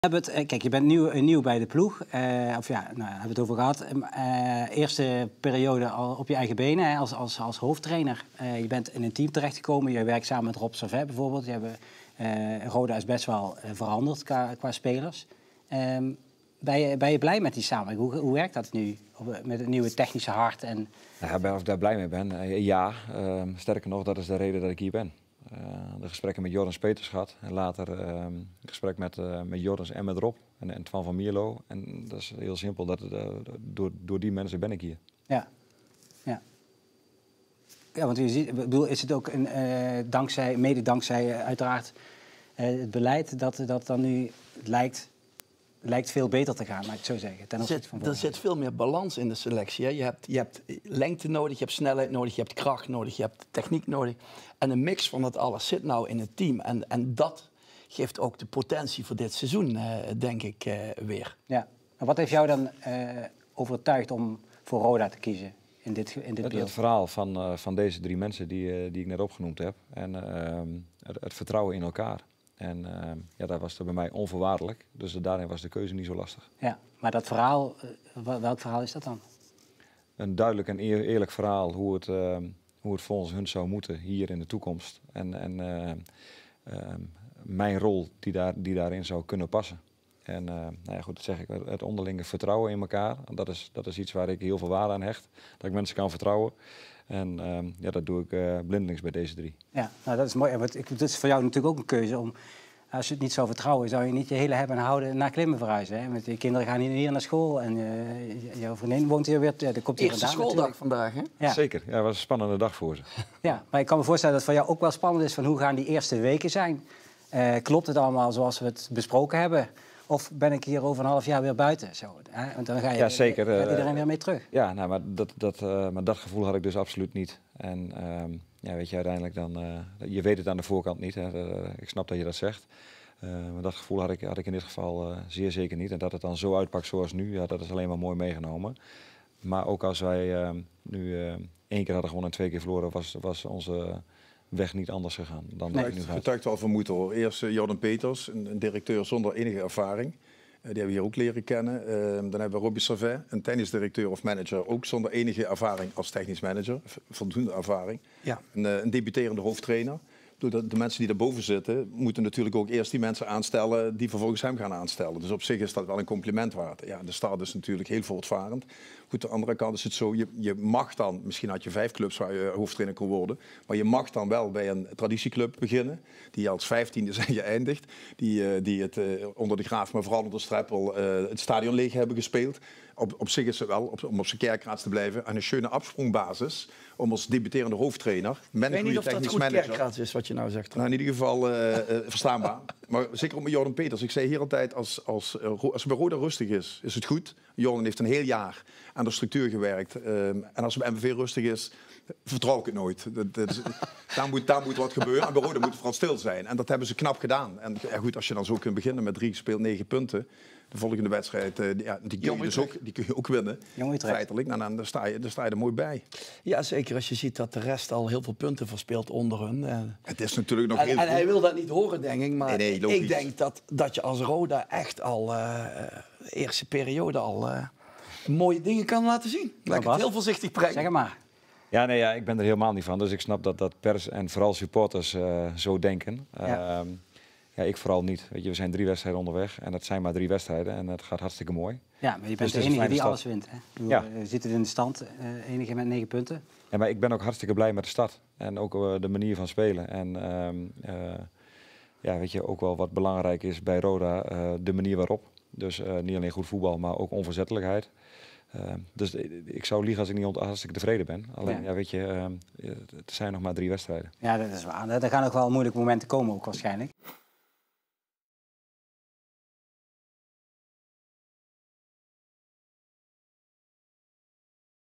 Maurice Graef prijst aanpak Bas Sibum: 'Supporters willen niks liever' | 1Limburg
Maurice Graef prijst de aanpak van Bas Sibum om betrokkenheid te creëren met de supporters.